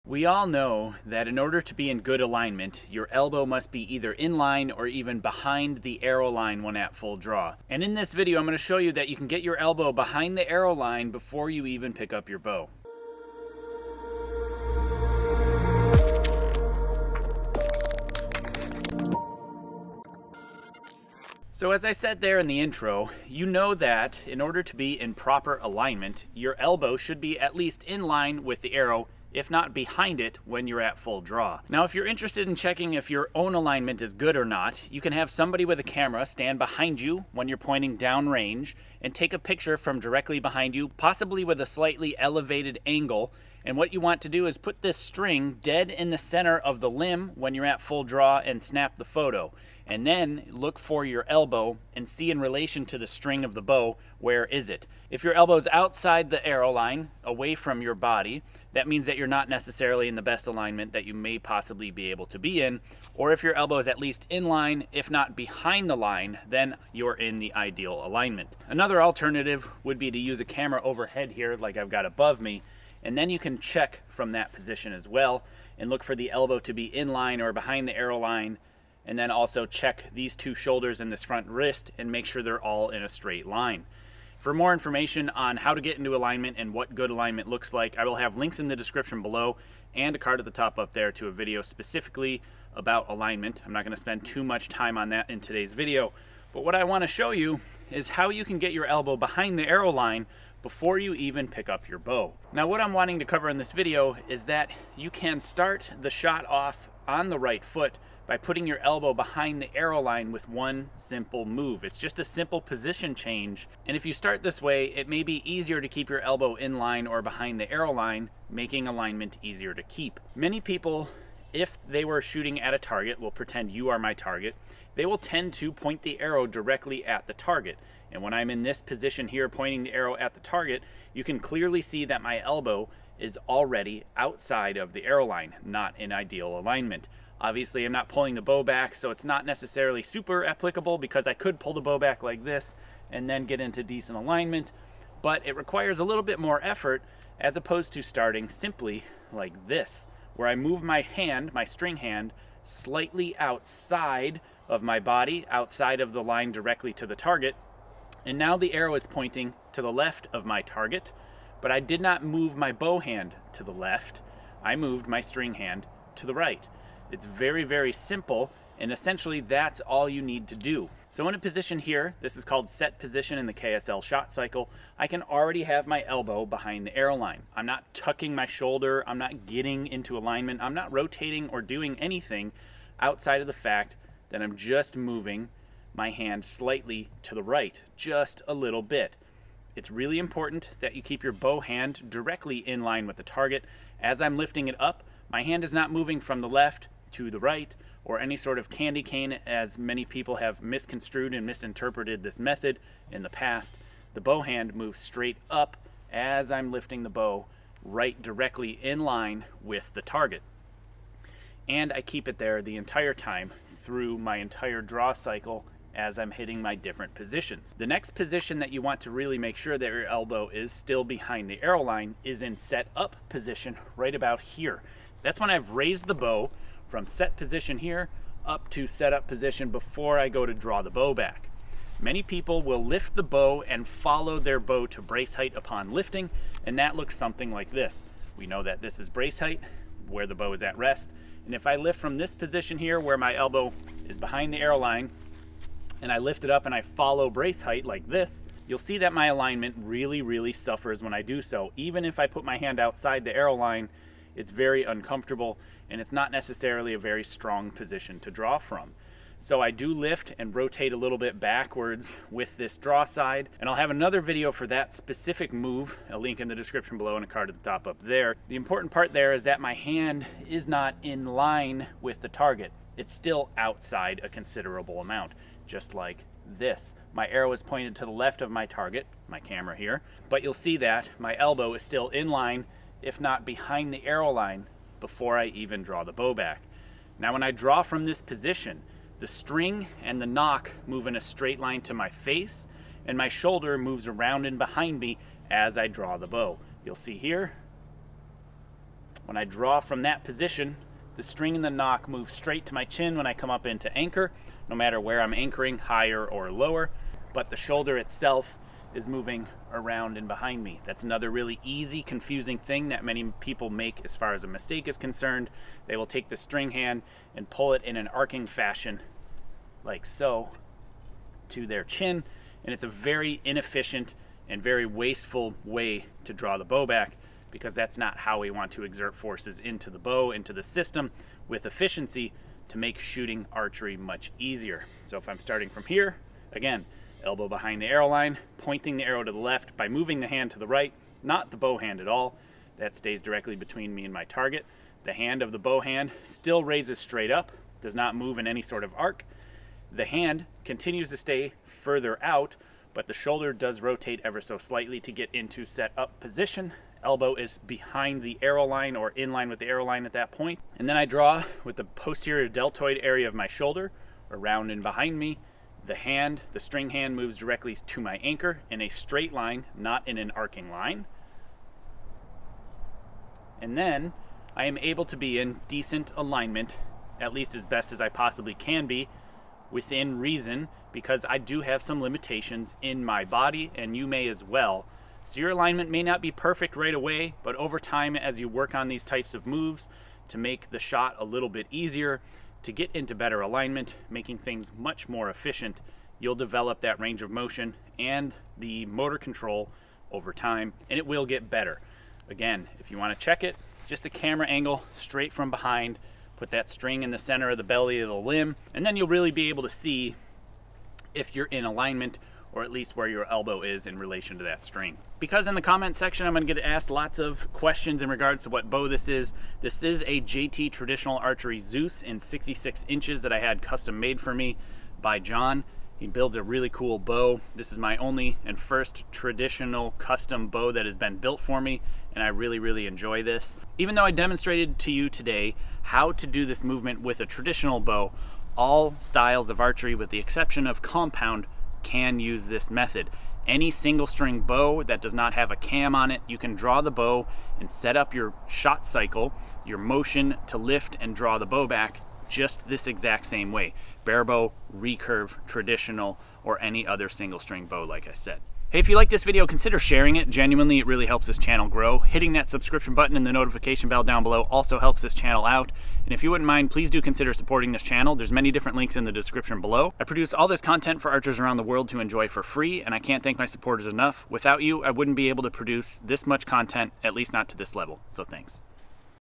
Make Archery Easier! Get Into Alignment As Soon As Possible!
Are you in good alignment when shooting your bow? How can you tell if you are in good alignment when shooting archery? Did you know you can get into alignment before even drawing your bow back? In this video, I show you how you can get your elbow behind your arrow line before you even draw your bow back.

Chapters:
00:00-00:23 Intro
00:23-1:42 How to check your Archery Alignment
1:42-3:37 Get into Alignment in Set Position
3:37-4:46 Set-Up Alignment
4:46-5:35 Hand and Nock Movement During the Draw
5:35-6:14 In a Nut Shell
6:14-8:00 Individual Limits

Send Cool Stuff To:
Jake Kaminski